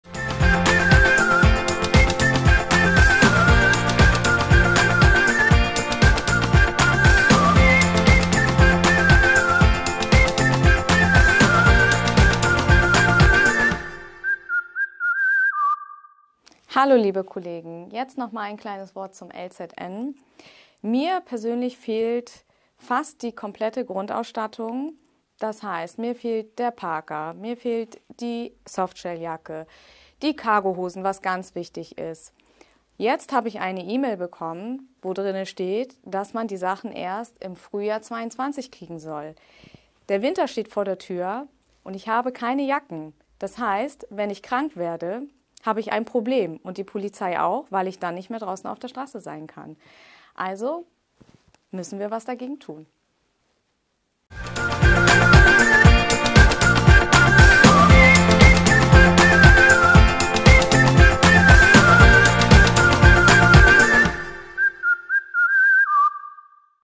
DPolG Hamburg - ...des Kaisers neue Kleider...
Hat das LZN (Logistik Zentrum Niedersachsen) das Märchen von  Hans Christian Andersen in das 21. Jahrhundert geholt?

Die neuen Kolleginnen und Kollegen der Verkehrsdirektion 1 sind nun seit Juni 2021 im Dienste der Polizei Hamburg. Viele persönliche Ausrüstungsgegenstände sind jedoch gar nicht oder nur unvollständig vorhanden.
So müssen viele Kolleginnen und Kollegen seit Wochen auf Hosen und Jacken verzichten und tragen zum Teil sogar private Hosen, um ihren Dienst für die Polizei Hamburg leisten zu können.
Dies kann aber bereits in den nächsten Wochen vorbei sein, denn auch die Winter- oder auch Übergangsjacken (Parka und Softshelljacke) sind derzeit nicht lieferbar.

Laut E-Mail vom LZN, die der Redaktion vorliegt, ist mit einer Lieferung der Hosen und der Jacken nicht vor Februar 2022 zu rechnen.

Wird das  Hans Christian Andersen Märchen in Hamburg bald wahr?

© Alle Videos by Michael Adomat